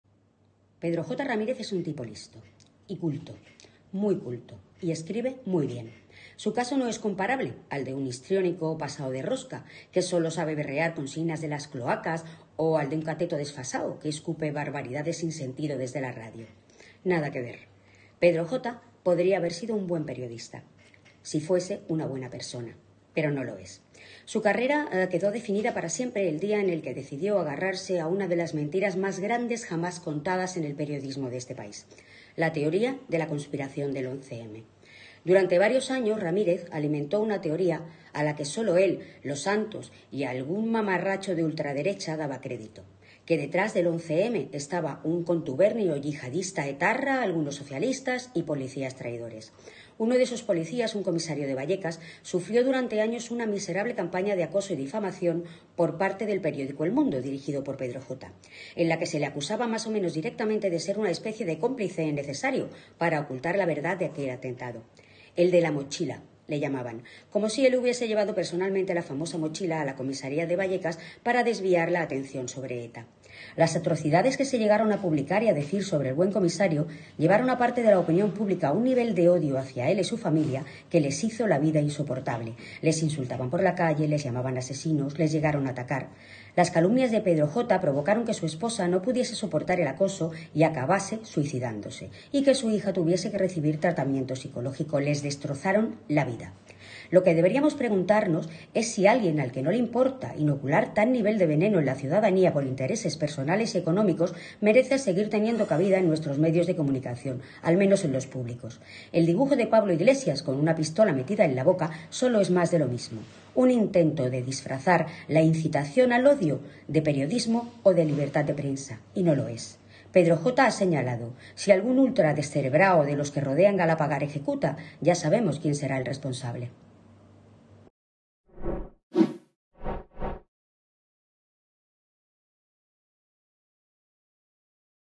OPINIÓN: Para ser buen periodista, hay que ser buena persona | Protestona
En el vídeo de hoy, Protestona analiza la figura de Pedro J Ramírez, un tipo "listo y culto" no comparable a los que solo saben "berrear" consignas "de las cloacas" o al de "un cateto desfasado que escupe barbaridades sin sentido desde la radio".